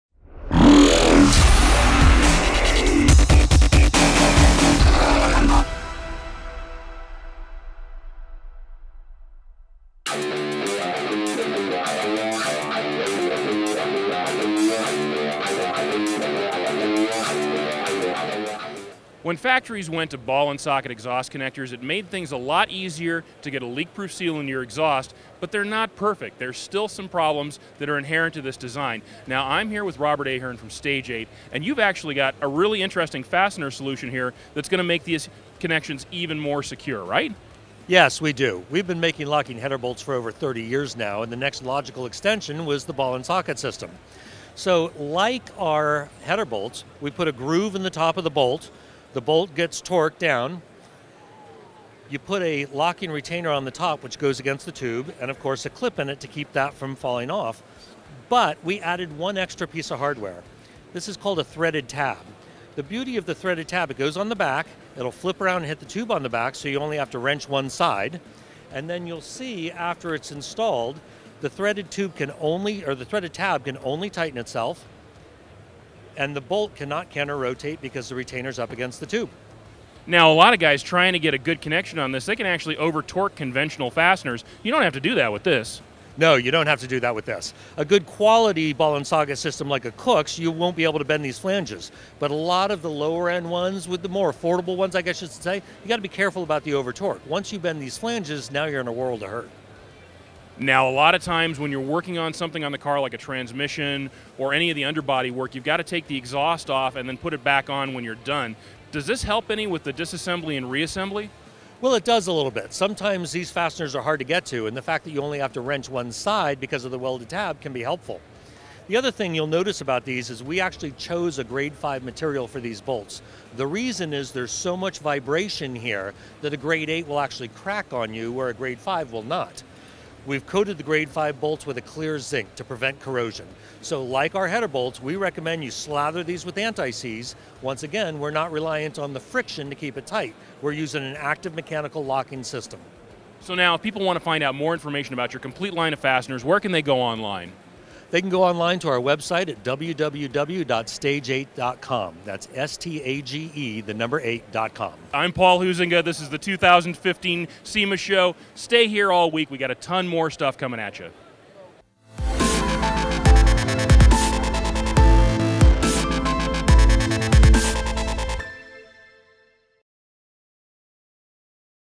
SEMA 2015: Stage 8 Ball and Socket Exhaust Fasteners
When the manufacturers did away with gasketed header flanges and exhaust connections in favor of ball and socket designs, they made a huge leap forward in creating reliable seals. Stage 8 takes them to the next level with mechanically locked fasteners that don't rely on friction to maintain torque, so you don't need to bend a flange trying to keep things tight. Plus, their threaded tab design means you can install or remove them with a single wrench.